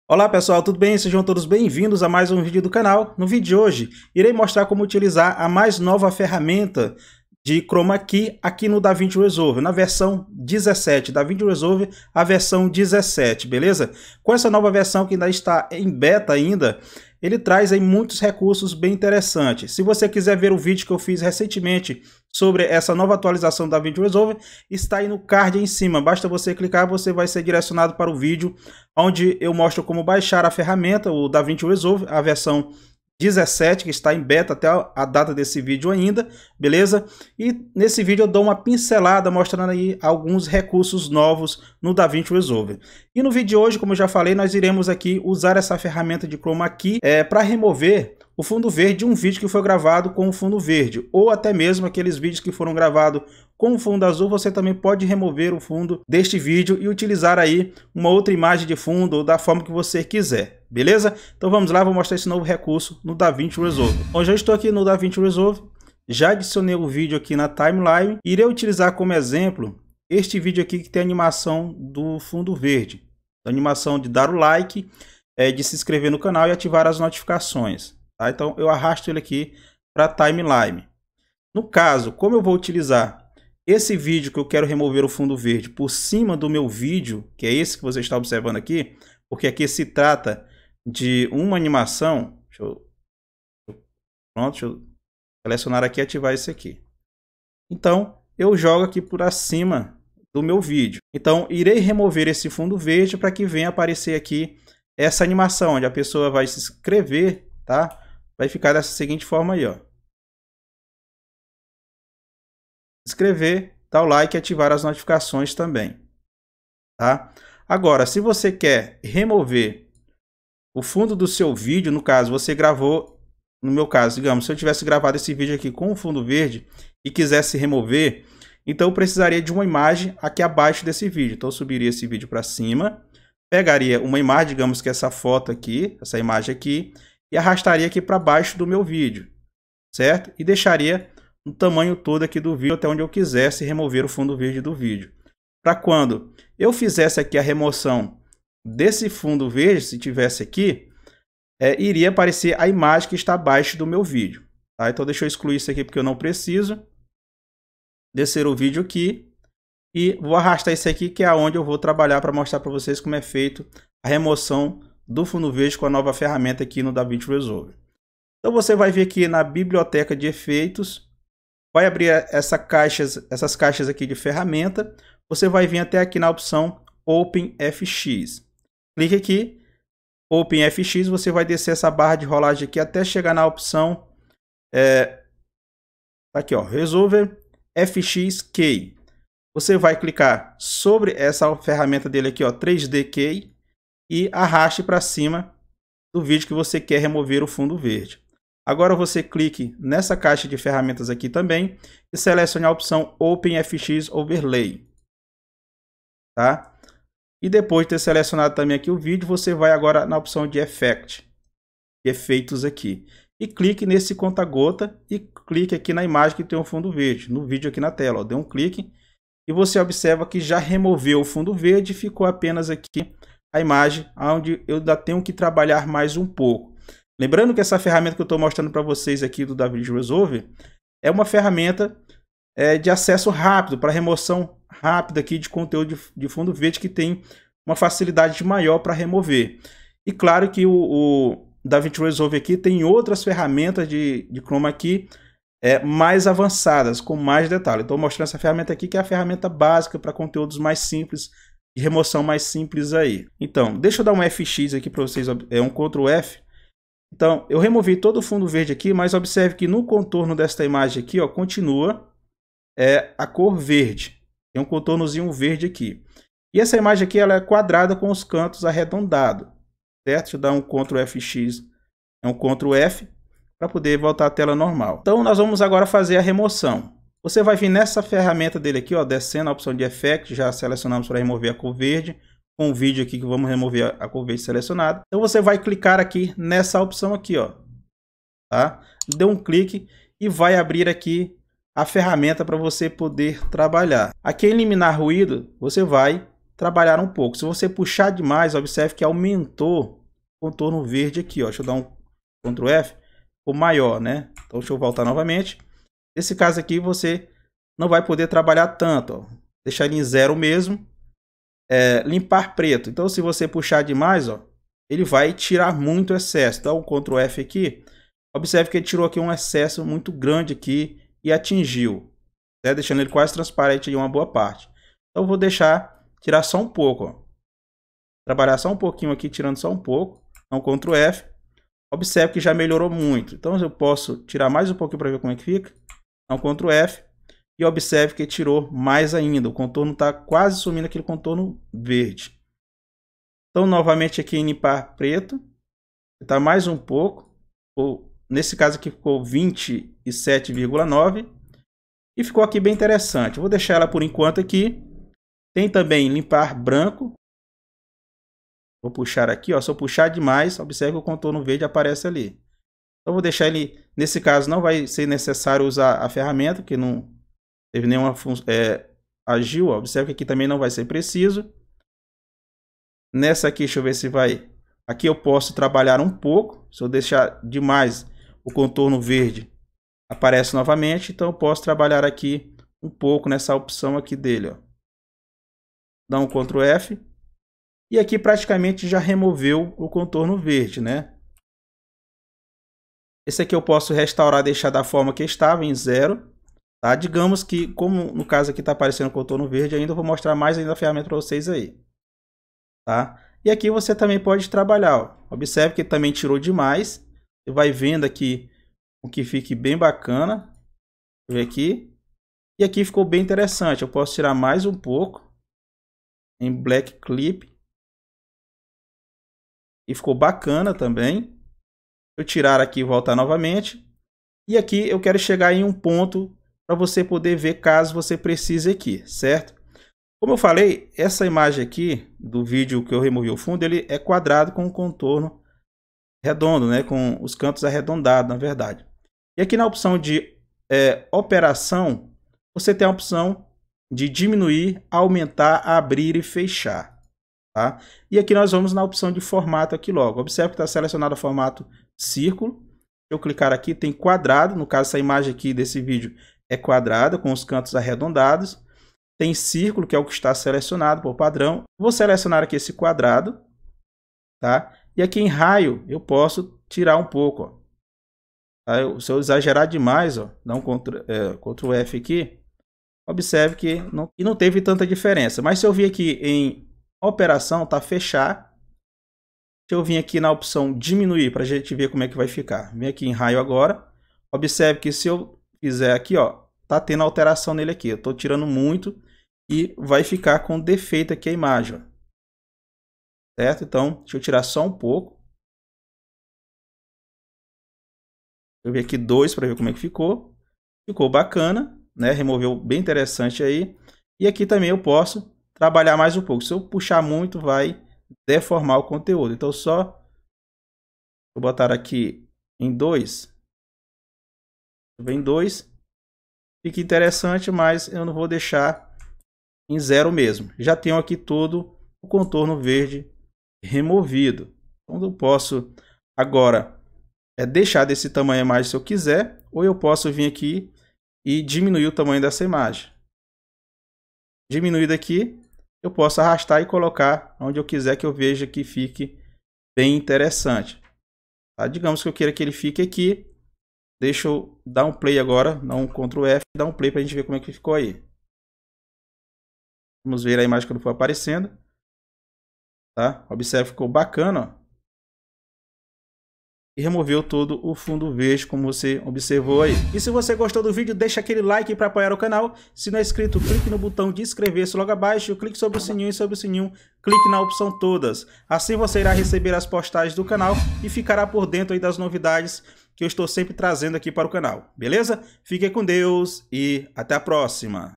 COMO REMOVER FUNDO DE CHROMA KEY | Novo Davinci Resolve
Olá neste vídeo mostro Como remover o Fundo de Chroma key no Novo Davinci Resolve 17, é isso mesmo remover o fundo verde ou azul do vídeo. É o que você vai ver hoje nesse tutorial, espero que gostem.

Recentemente foi disponibilizada ao público a versão “Beta” do DaVinci Resolve 17. O editor de vídeos da “Blackmagic Design” está chegando com várias novidades e melhorias tanto para a sua variação paga quando para a gratuita.

INDICAÇÕES DE MAIS VÍDEO

Playlists DaVinci Resolve.

Playlist Unboxing | Review.

Playlist Shotcut.

Playlist Kdenlive.

Playlists OBS Studio.

Playlist Programa | PC.

Grande abraço